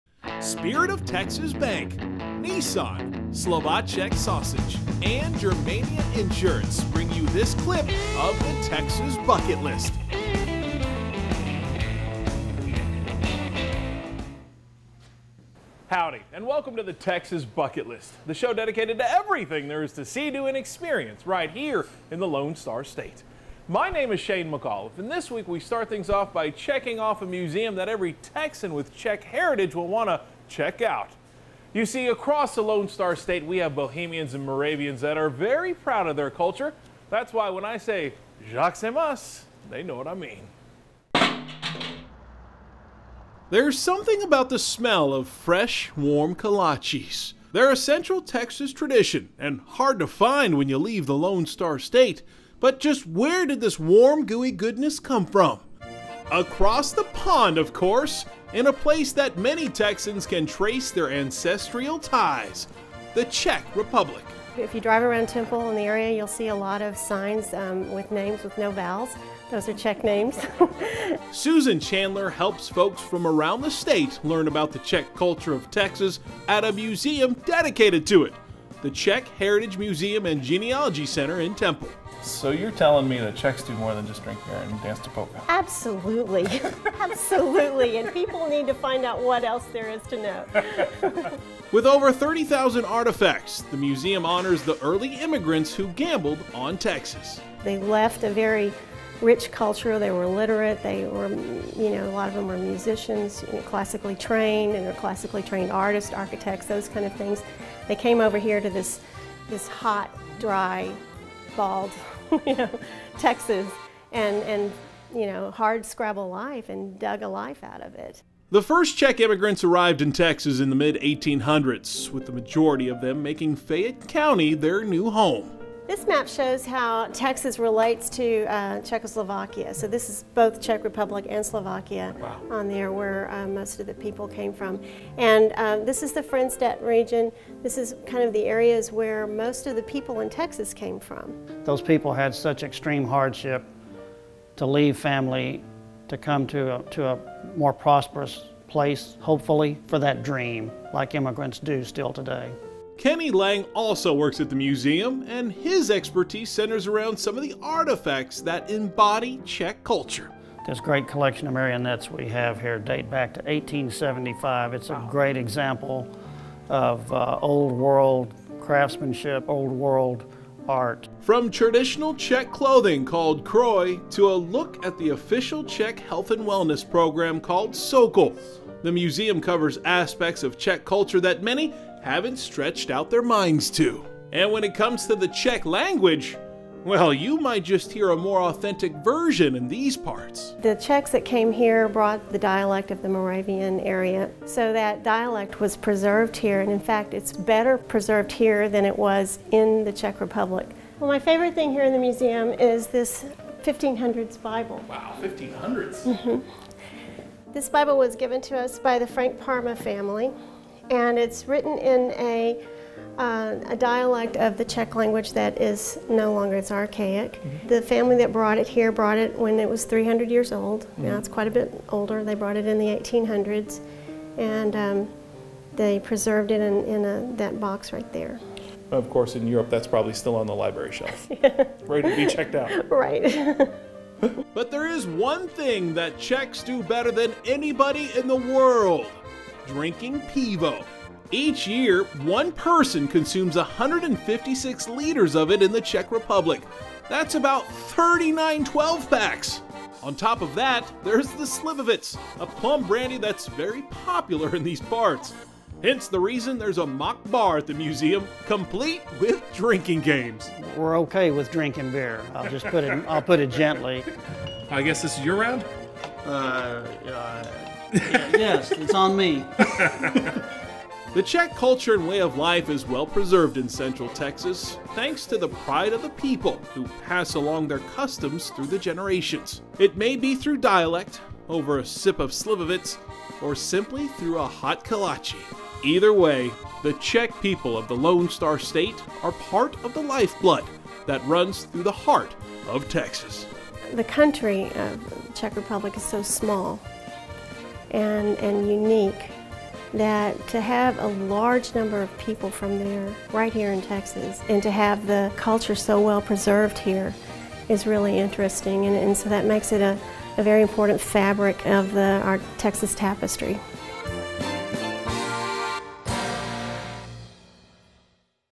The Texas Bucket List - Czech Heritage Museum and Genealogy Center
Any Texan with Czech heritage is going to want to check out this museum!